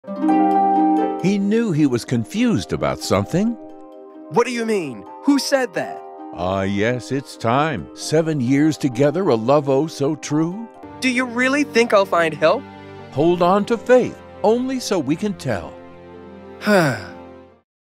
This is a short example of a two-character narration, where I am doing both roles thanks to AI
What if you could do the voices of multiple characters in a narration, but not all in your own voice?  I've found a service that lets me do all the acting, but alter the sound of my voice for each character.  I love it!  Here's an example.  I talk about it in the latest "Front Page News" newsletter.